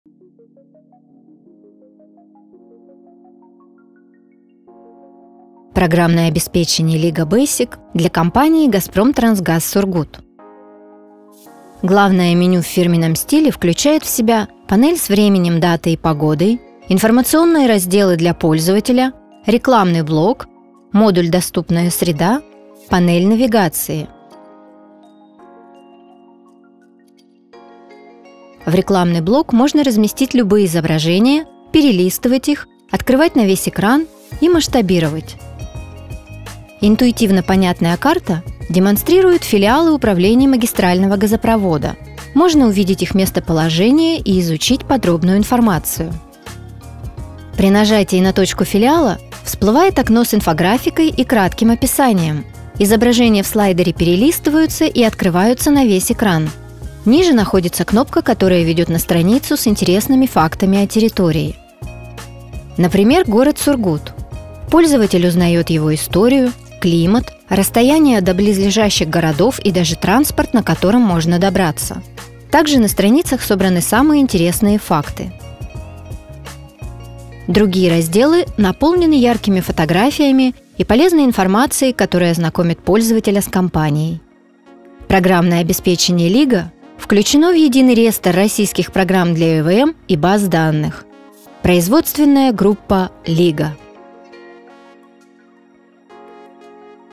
ПО (программное обеспечение) Газпром Трансгаз Сургут
Программное обеспечение Liga Basic разработанное для компании “Газпром трансгаз Сургут” представлено в фирменном стиле. В программном обеспечении включены модули: погода, время и дата, доступная среда, рекламный блок, информационные разделы для пользователей. Карта с точками местоположения всех филиалов магистрального газопровода отображает всю информацию о месте, фото с кратким описанием.  Данное ПО включает в себя интересные факты о территории, начиная от исторически значимых памятников и заканчивая транспортом, на котором можно доехать до близлежащего населенного пункта.

ПО «Liga Basic» (Лига Бейсик) включено в Единый реестр российских программ для ЭВМ. Программное обеспечение внесено в реестр российских программ (реестровый №12474 от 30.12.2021). Оборудование LIGA занесено в реестр МИНПРОМТОРГА, лицензировано и сертифицировано, поставка осуществляется по 44-ФЗ и 223-ФЗ.